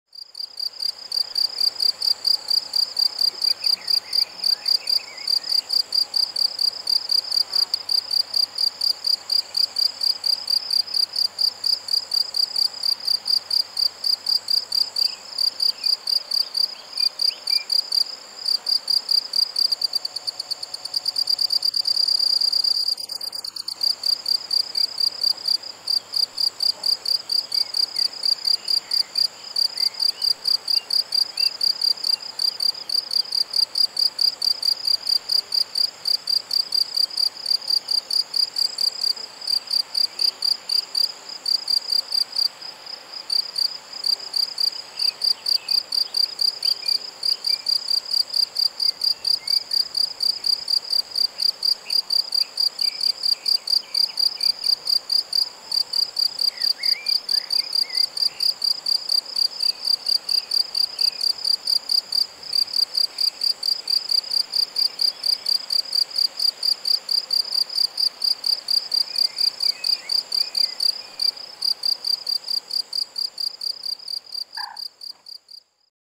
LIBELLULE Déprimée/Libellula depressa/Mâle ! BRUITX
Observations et Enregistrements au bord d'une mare dans la campagne du centre de la France : Le 5 Mai 2020 ! Disponible en 2K !!